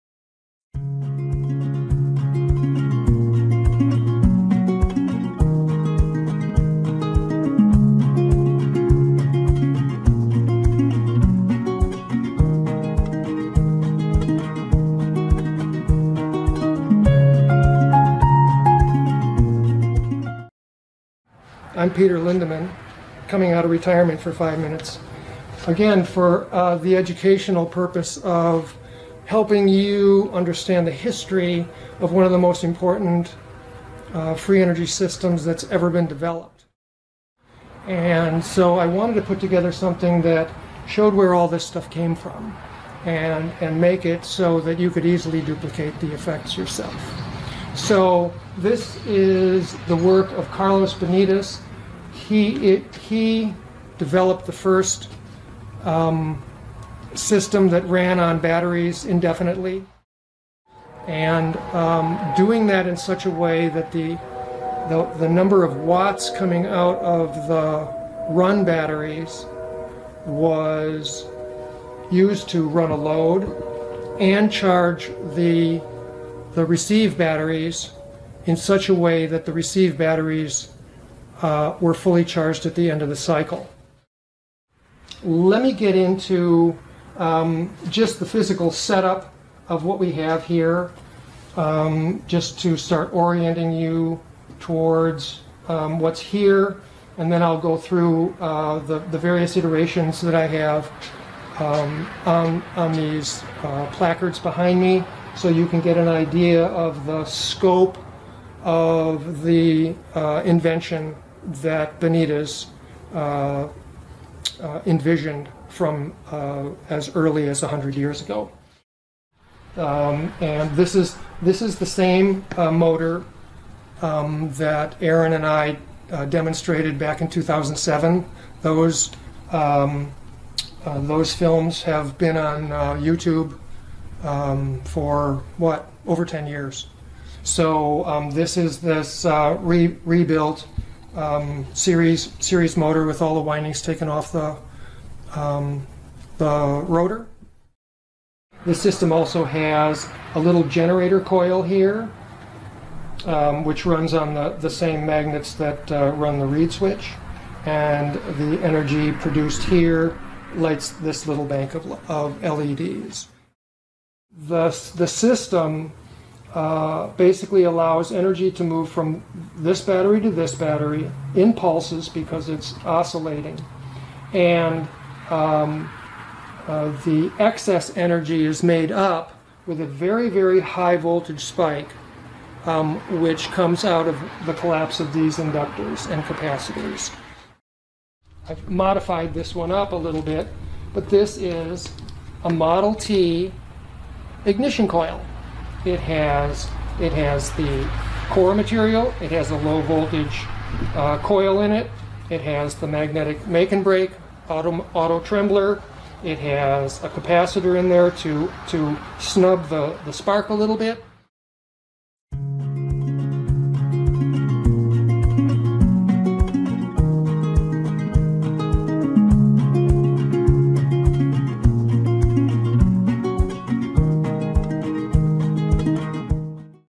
Self-recharging battery with Peter Lindemann 2018 Energy Science & Technology Conference
In the early days of the 'electrical era' many experimenters were claiming new ways to produce useful electrical effects, but Benitez was able to run light bulbs and motors from batteries that never needed to be re-charged from a conventional, external source. Peter Lindemann demonstration.

For dozens of videos, live calls, courses, books and manuals in an environment with other alternative DIY engineers, join The Clean Energy Academy